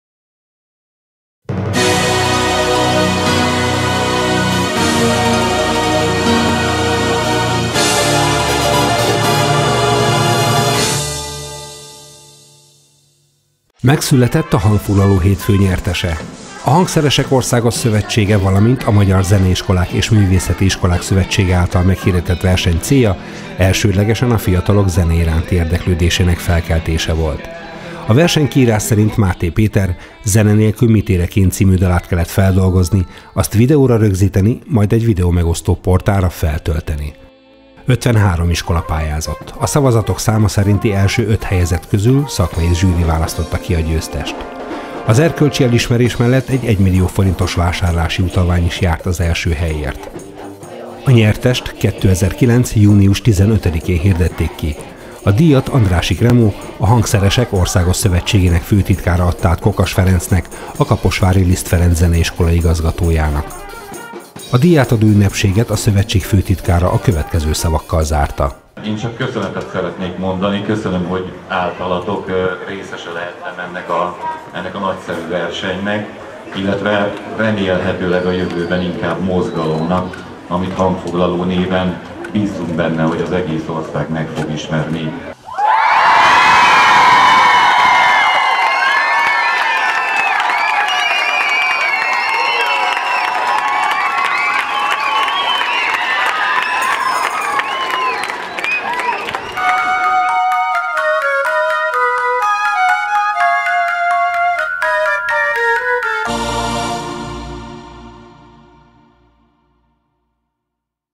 Hangfoglalás Híradó - Kaposvár
A Hangfoglaló Hétfő díjának átadása Kaposváron 2009-ben